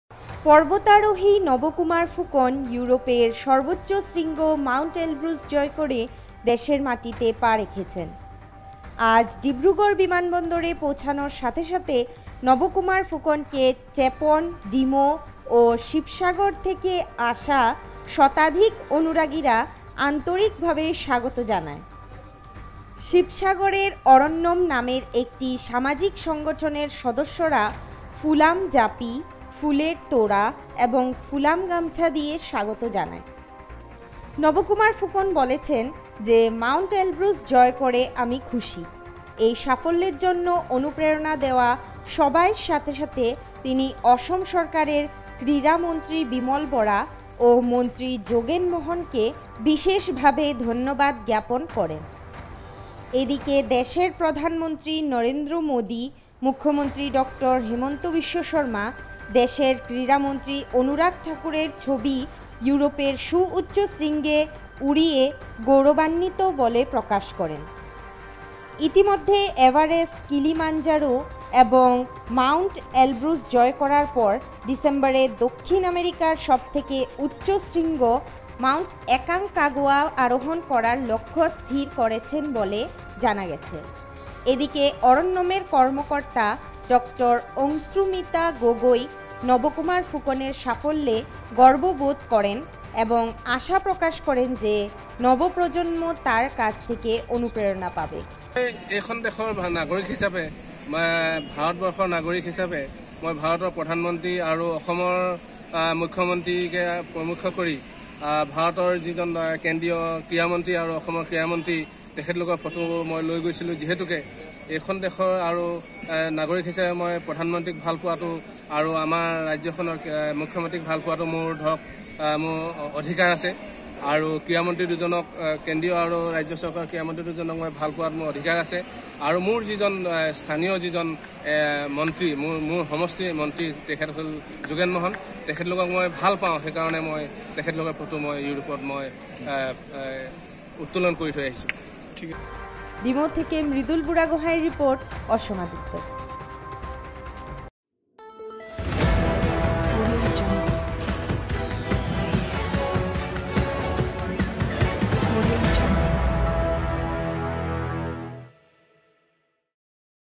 NABA KUMAR PHUKAN
ইউরোপের সর্বোচ্চ শৃঙ্গ মাউন্ট এলব্রুস জয় করে দেশের মাটিতে পা রাখলেন পর্বতারোহী নব কুমার ফুকন।